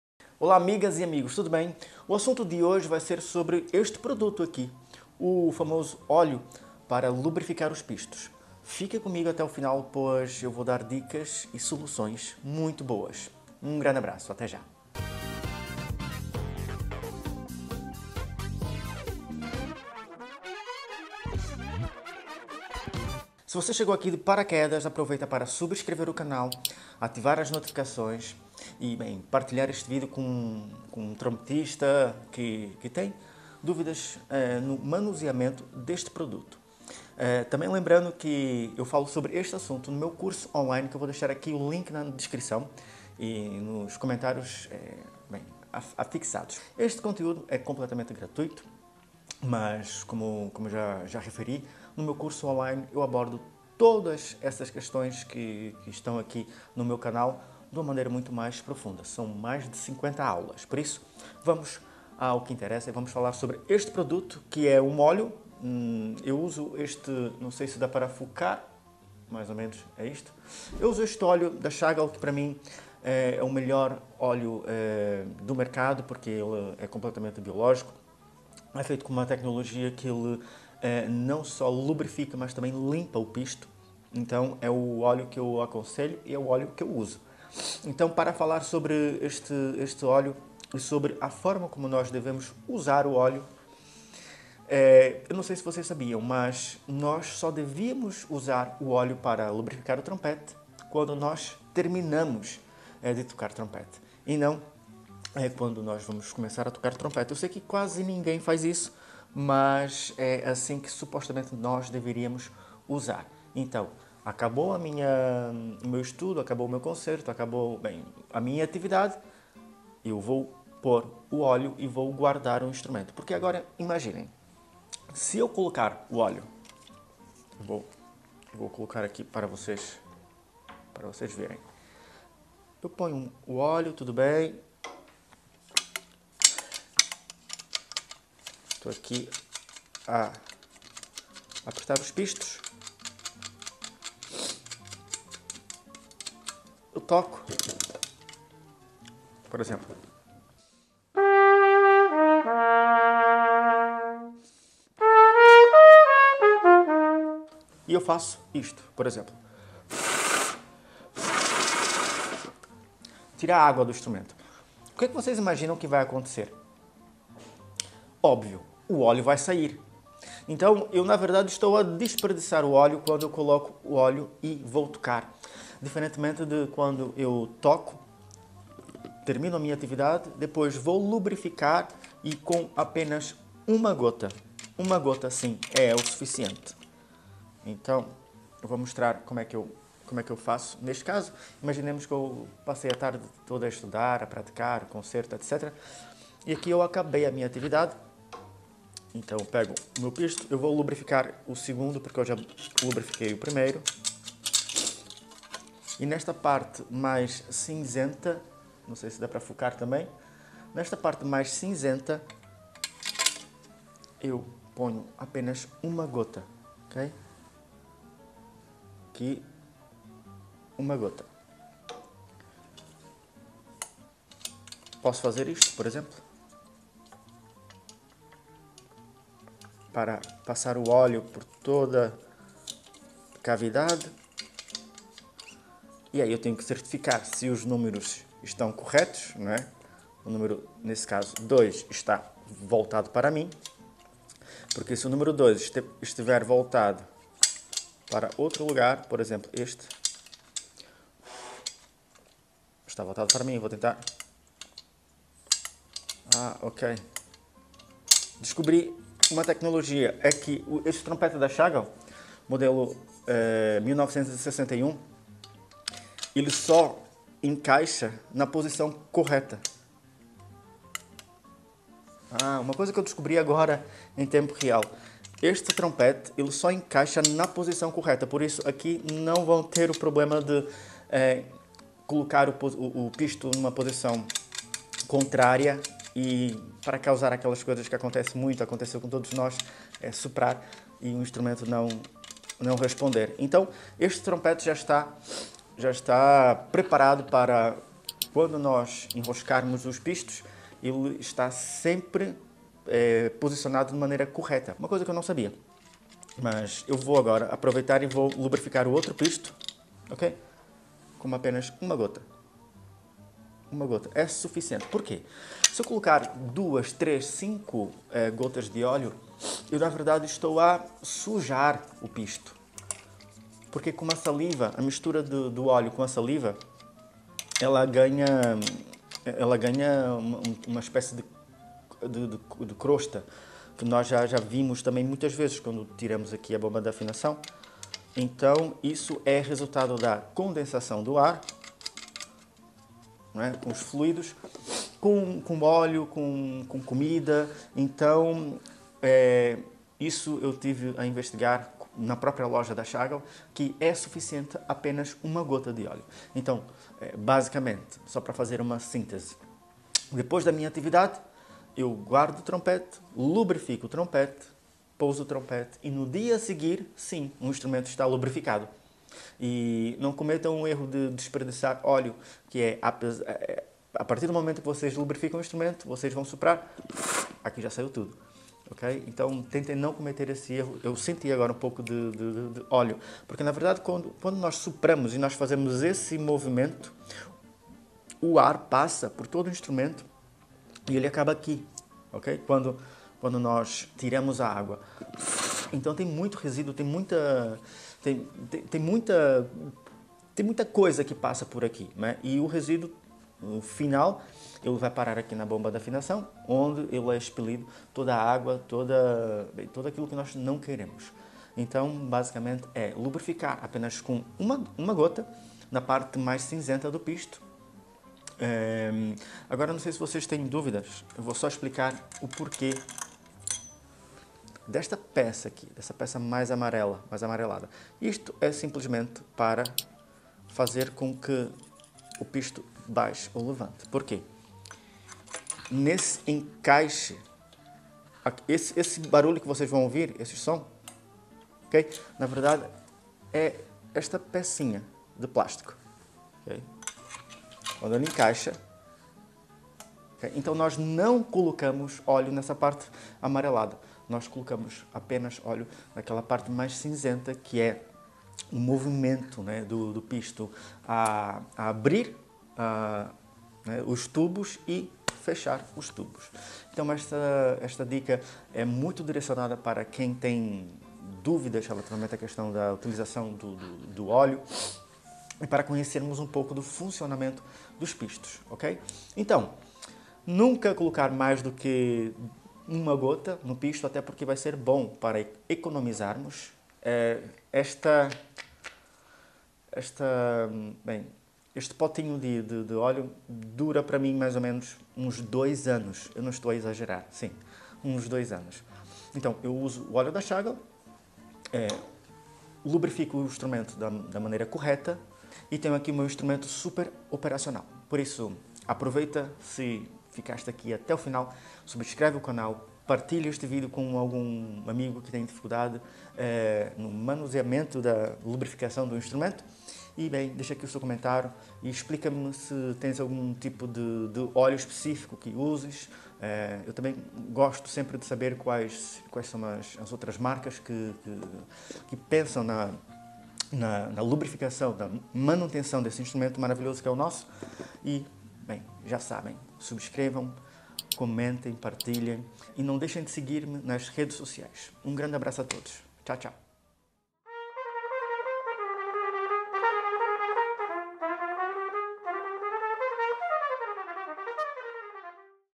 VOCÊ SABE LUBRIFICAR O TROMPETE CORRETAMENTE?
Neste vídeo, vamos aprender como lubrificar corretamente os pistões do trompete, garantindo que seu instrumento funcione de forma suave e precisa. Se você é trompetista e tem dúvidas sobre como aplicar o óleo corretamente, este tutorial é para você! Vou mostrar um passo a passo simples e eficaz para manter os pistões do seu trompete em ótimas condições e prolongar a vida útil do instrumento.